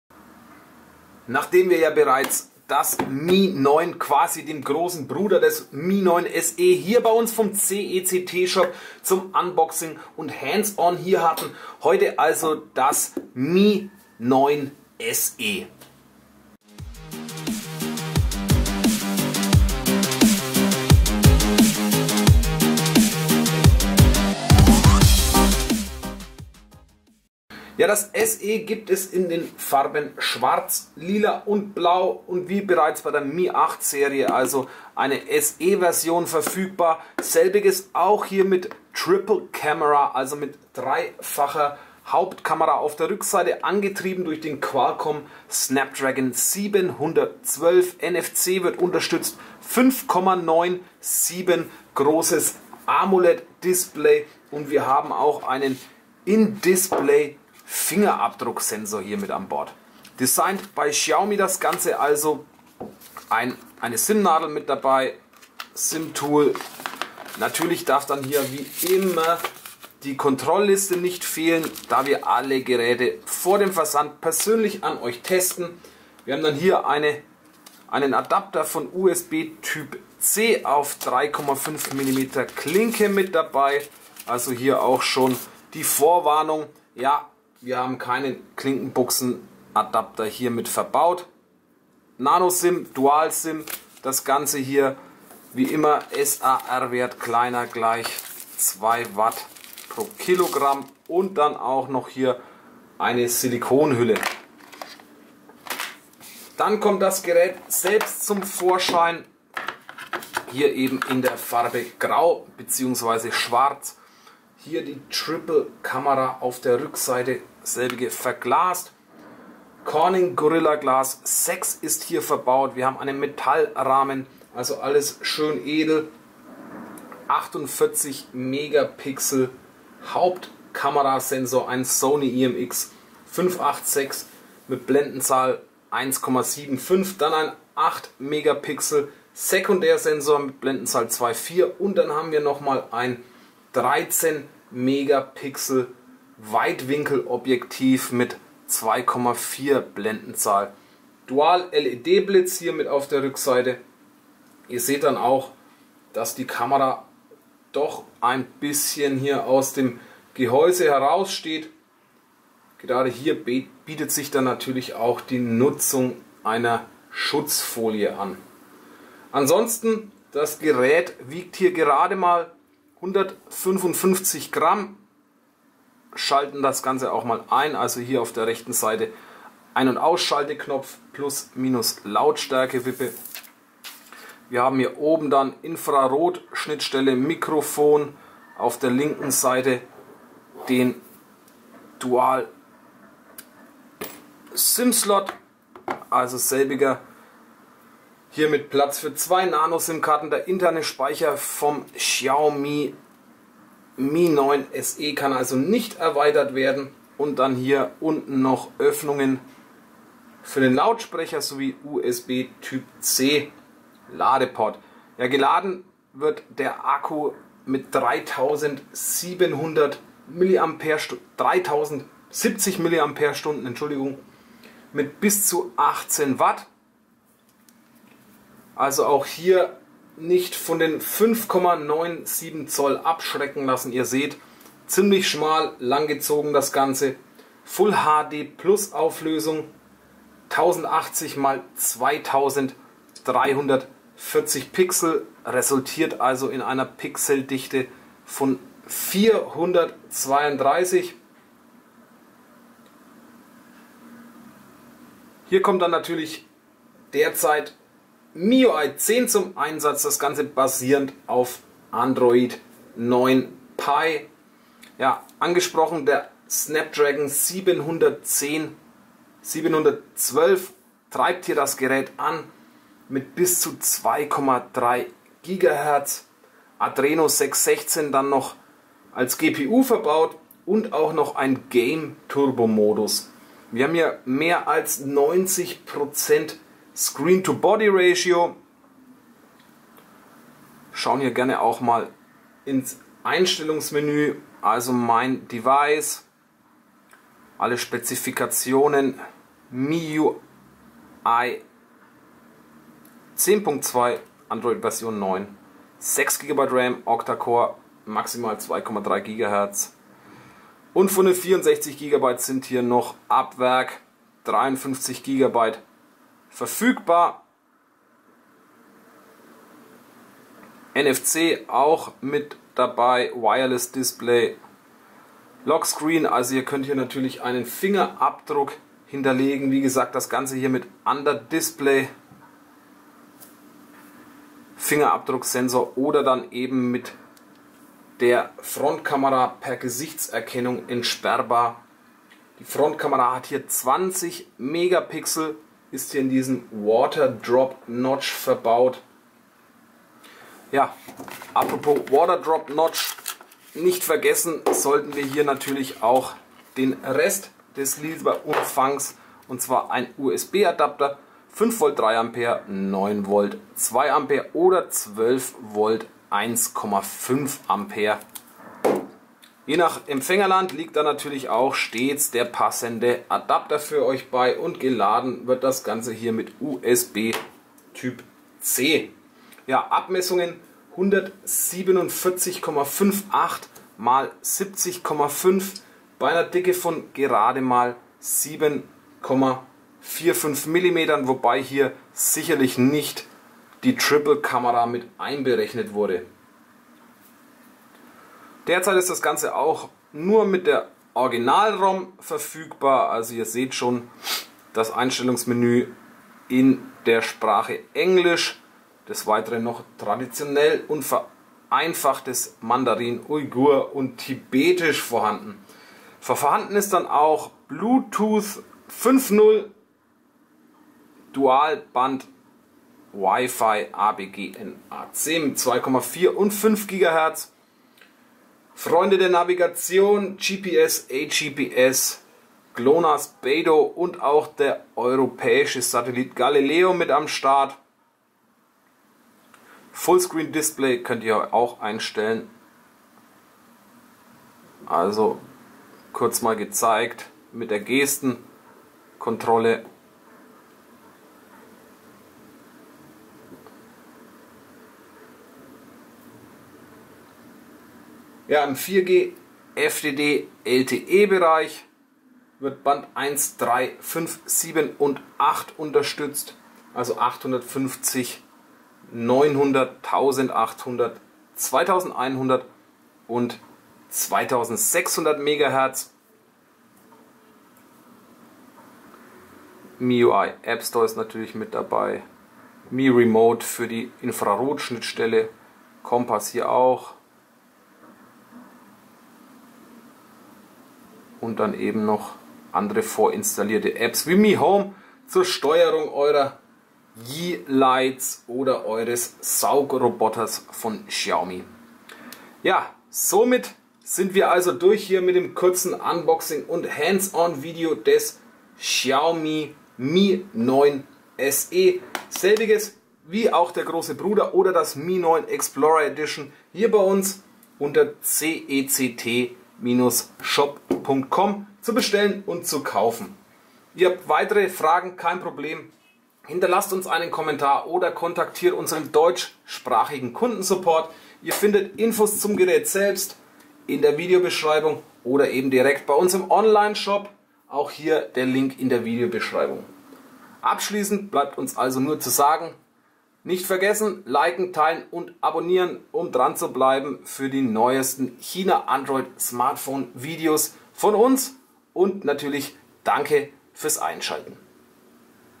Xiaomi Mi 9 SE Unboxing & Hands-On | CECT-Shop.com
CECT-Shop.com - Dein #1 Online-Shop für Android Smartphones, Handys, Tablets, Gadgets & More

• 5,97" Super AMOLED Display
• In-Display Fingerabdrucksensor + Face ID
• Speicherkapazität: 64 GB / 128 GB ROM
• 64-Bit Qualcomm Snapdragon 712, Octa Core, 2.3 GHz
• Triple-Kamera: 48 MP + 8 MP + 13 MP
• 20 MP KI Frontkamera
• NFC, Smart PA, Bluetooth 5
• 3070mAh Akku (18W)
• WiFi Display, WiFi Direct, 802.11a/b/g/n/ac
• Kein SIM-Lock / Kein Vertrag

► Folge uns!